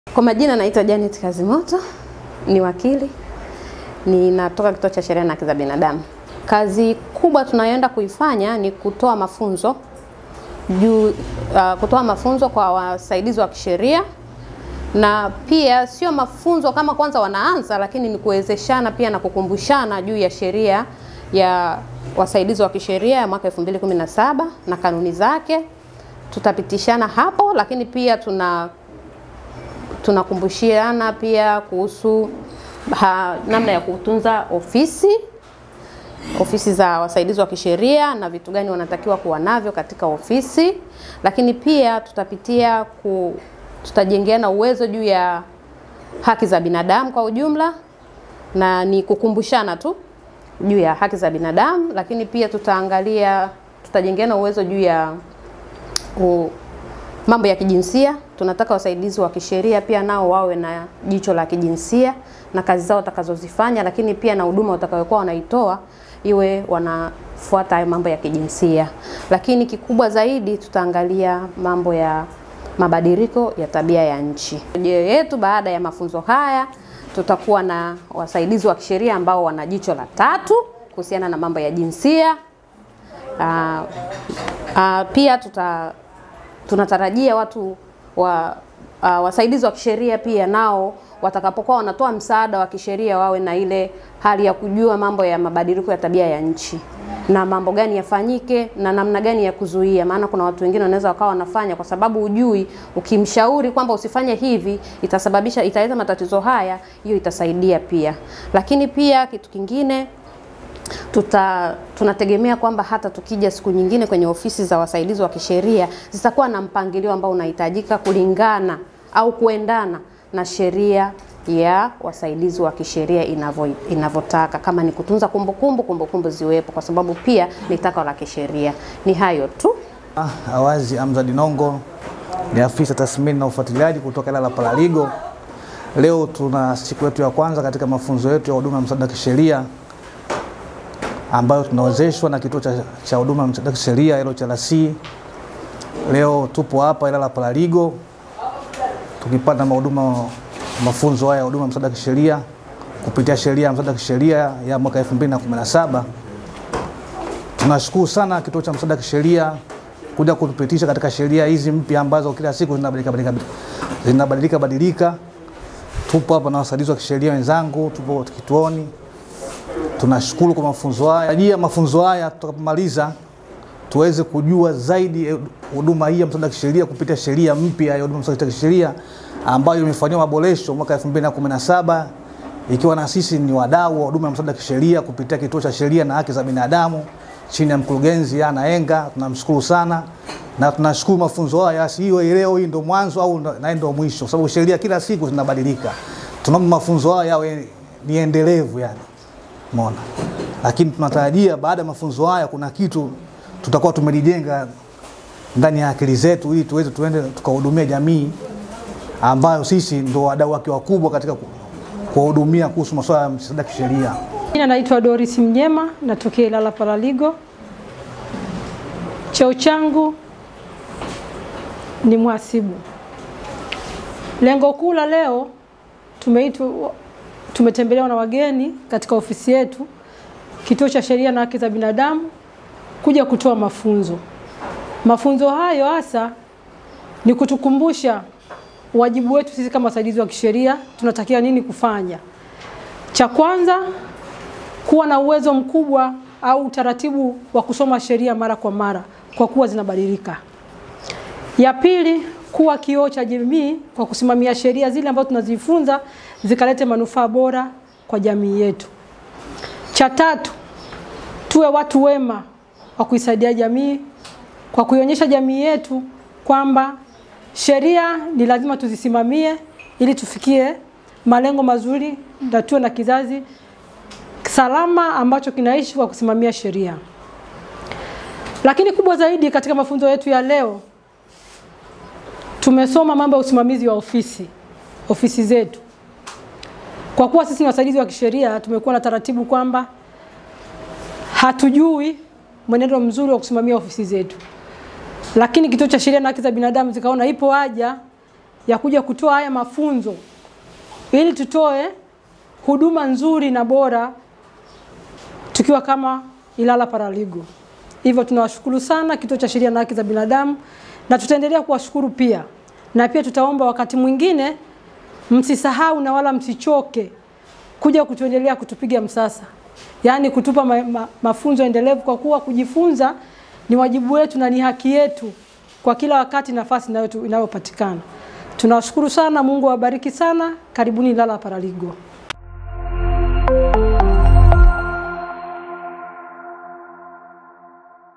Wasaidizi wa Msaada wa kisheria ILAP wapongeza mafunzo ya LHRC
Kituo cha Sheria na Haki za Binadamu(LHRC) kimefanya mafunzo ya kuwajengea uwezo wasaidizi wa msaada wa kisheria wilayani Ilala JijiniDar es Salaam.

Mafunzo hayo ambayo yanaendeshwa na maafisa sheria kutoka LHRC yamelenga juu ya kuwapa elimu ya usimamizi wa ofisi na namna bora ya kuweza kuandaa taarifa mbalimbali za wananchi ambao wanawapatia msaada wa Kisheria.

LHRC inaendelea kutoa mafunzo na kuwajengea uwezo wasaidizi wa msaada wa kisheria katika mikoa mbalimbali nchini.